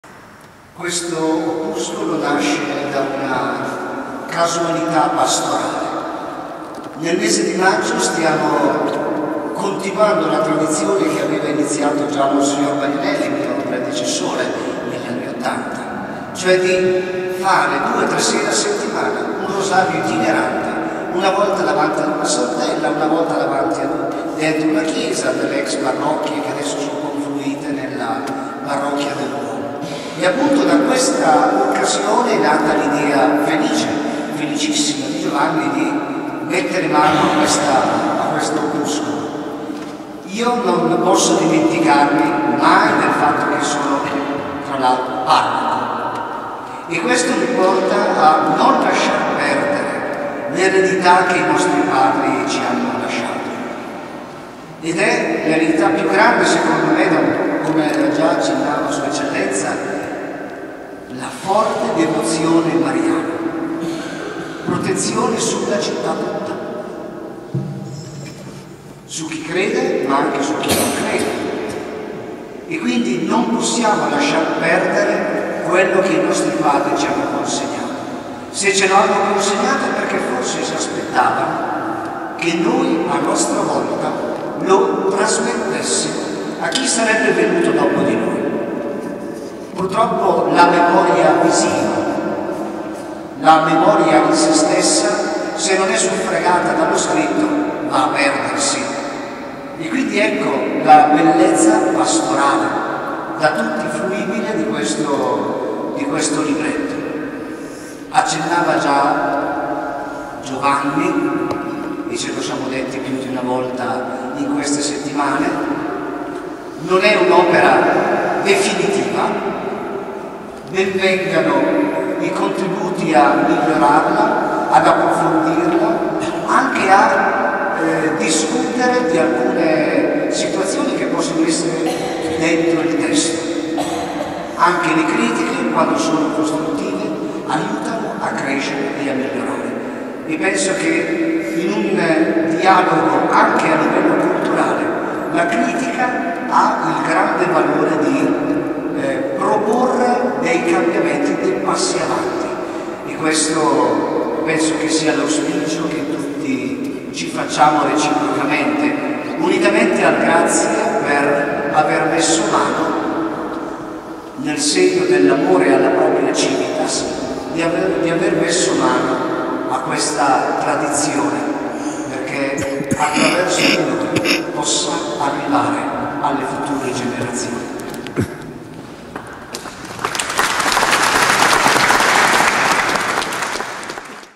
Edicole Votive di Urbino: don Andreas Fassa
Il parroco  della parrocchia Santa Maria Assunta della Basilica Cattedrale di Urbino Don Andreas Fassa, l'8 Novembre 2015 esprime il Suo parere sul libretto Giovanni Volponi sulle Edicole Votive sparse nel centro storico di Urbino.